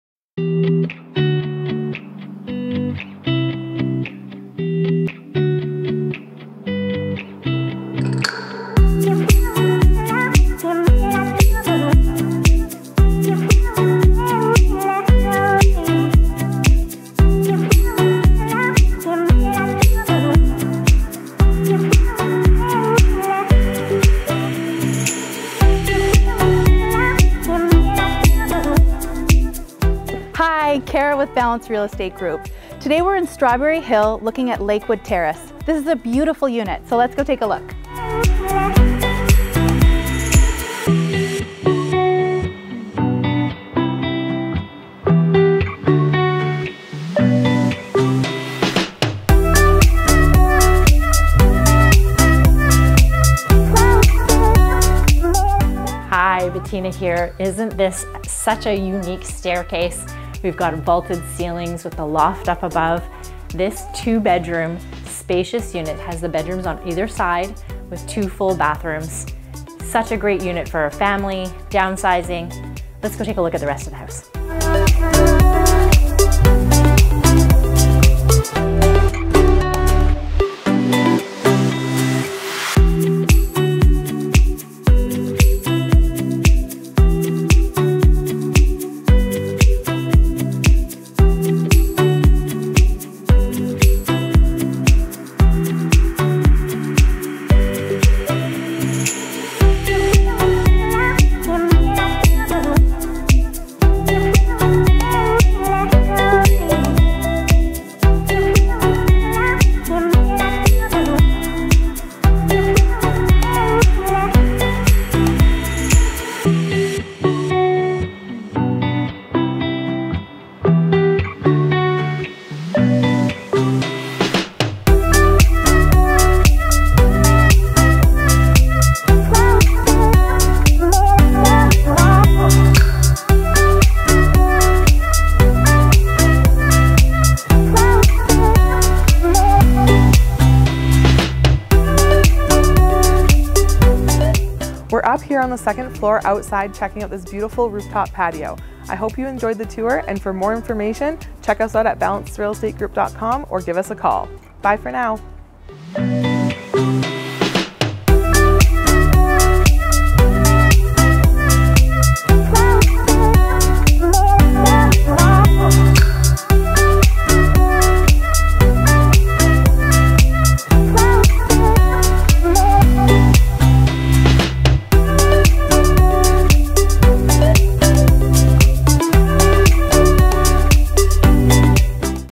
#301 6557 121 St,Surrey - Real Estate Video Tour - Balance Real Estate Group
View to see PRICE and more PHOTOS.

Rarely available 2 storey TOP floor unit in Lakewood Terrace. Walk into this over 1300sf unit. To spacious dining room and kitchen featuring Stainless Steel fridge and stove. Plenty of cabinets and counter space. The laundry room off the kitchen offers storage and a BRAND new washer and dryer. 2 bedrooms on opposite sides of the unit. Spacious master bedroom, large enough to fit a king-sized bed. Vaulted ceilings in the living room with a spiral staircase leading up to the second floor. South facing deck off the living room offers plenty of sunshine all day long. Keeping the home bright. Large loft space perfect for a guest room, extra living room, or office space. Opens up to BONUS private rooftop deck to garden or suntan and read a book on. 2 underground parking. 1 pet allowed cat or dog. In an excellent location, only steps to Safeway, Tim Horton's, Starbucks, Doctors office with walk-in clinic, Dentists, restaurants & so much more.

Bedrooms: 2
Bathrooms: 2
SqFt: 1351